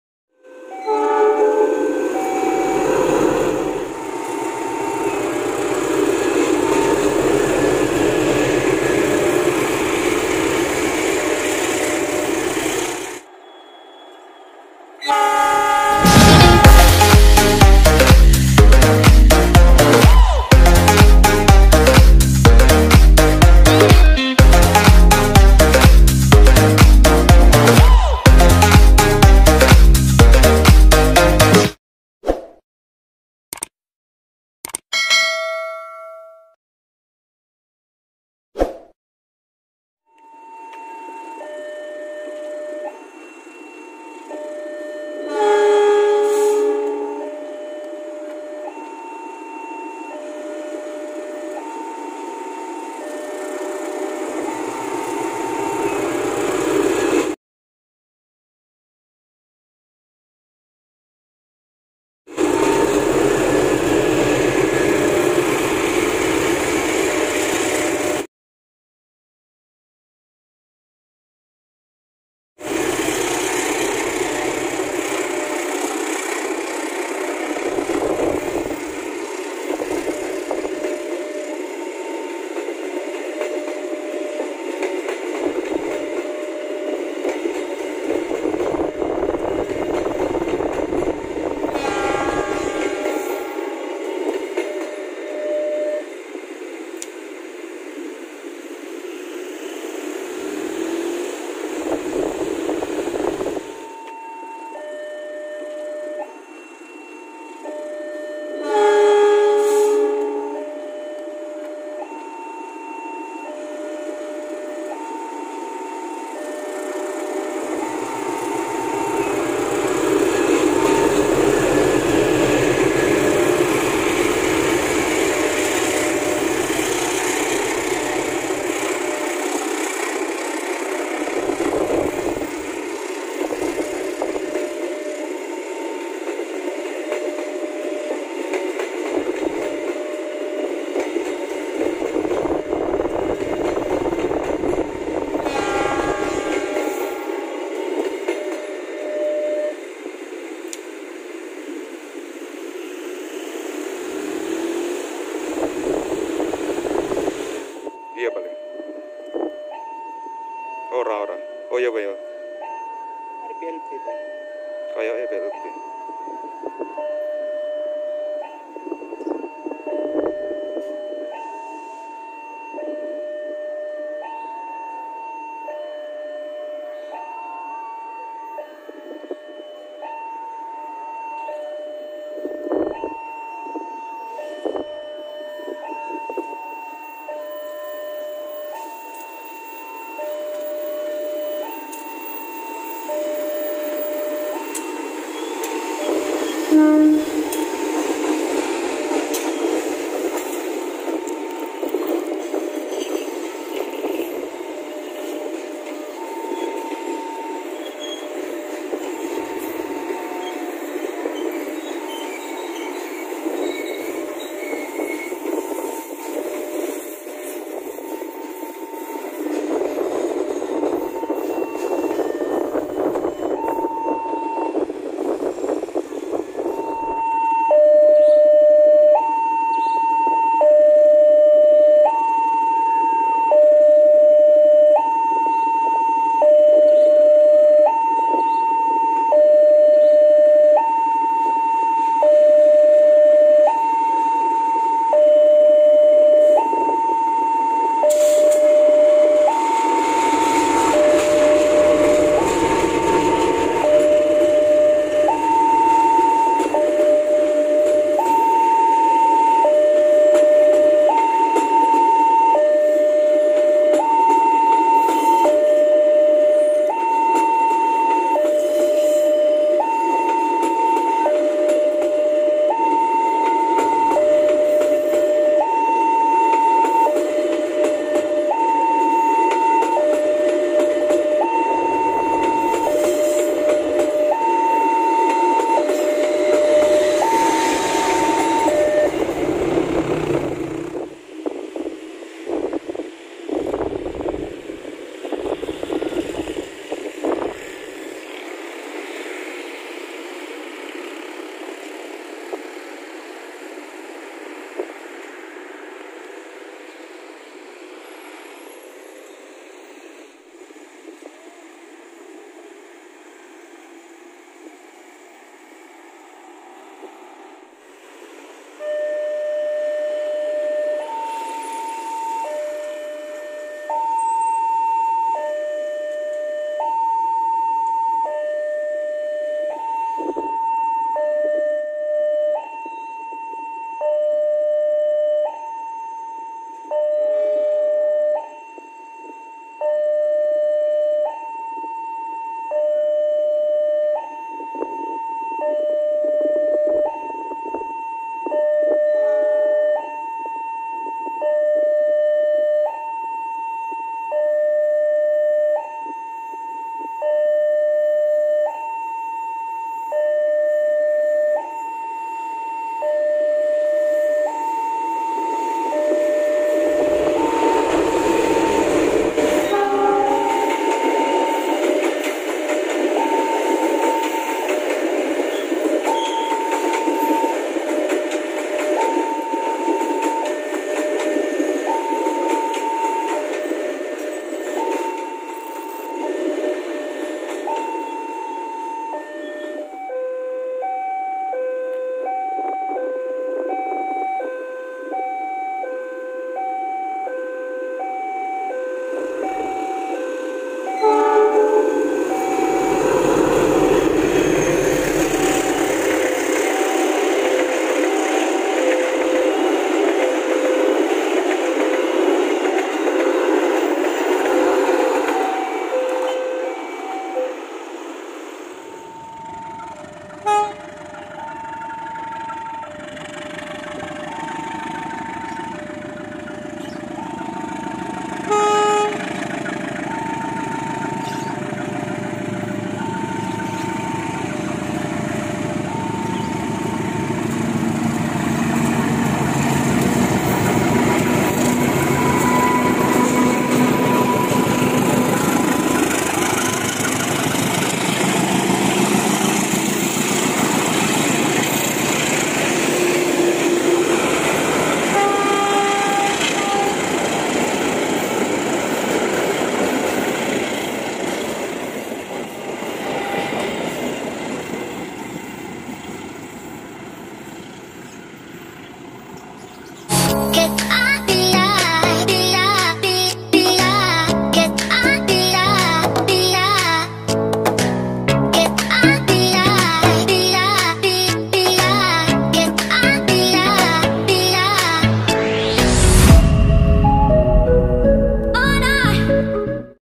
POLOSAN TANPA LOGO KAI!!! MASA PERALIHAN LOGO KAI YANG CUKUP UNIK PT KERETA API INDONESIA
Selamat Datang Kembali di Channel Saya
Hari ini saya upload video KA yang memakai logo polos sekaligus hunting KA Parcel Terlambat parah imbas anjlokan Serayu 1 Oktober kemarin
Seperti apa videonya? Monggo simak video ini sampai Selesai